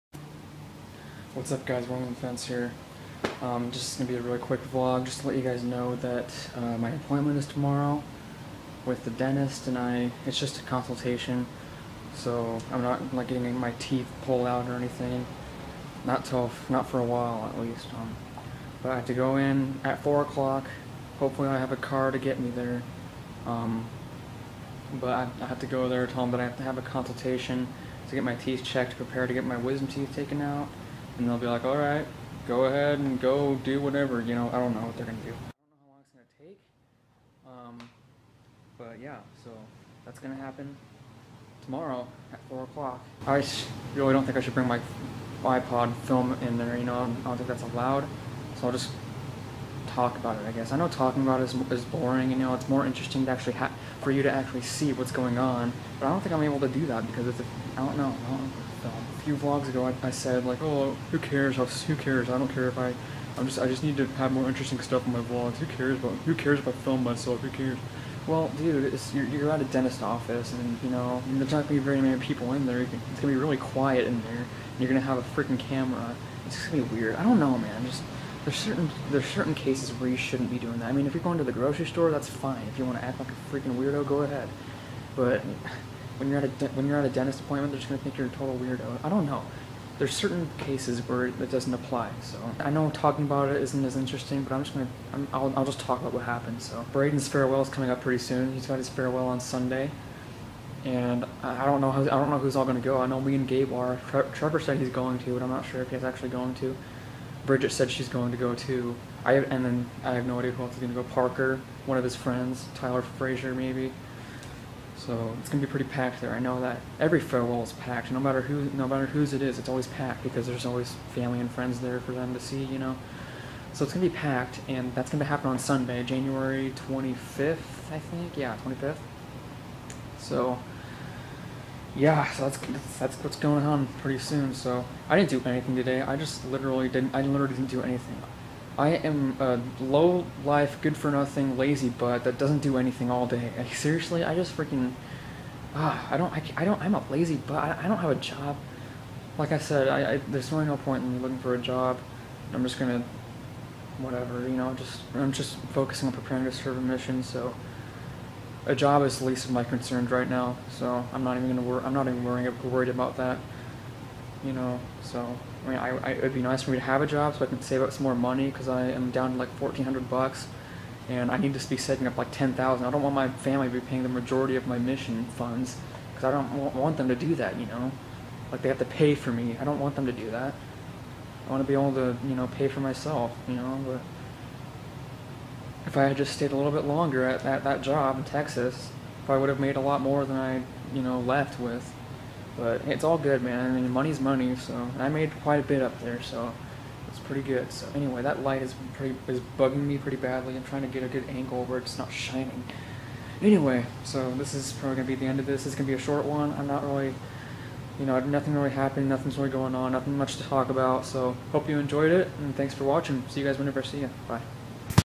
Tomorrow's Dentist Appointment (1/21/15) DAY 291
My rescheduled dentist appointment is tomorrow at 4:00 so I'm getting prepared for that. Also, Braden's farewell is in 4 days so we're planning on attending that as well. It'll be one of the last few times that I'll probably ever see him again, based on the fact that we're moving this year and when I get back from my mission in two years I'll be going home to Washington, but Braden will be in Idaho. I don't know though, but it's kind of crazy to think...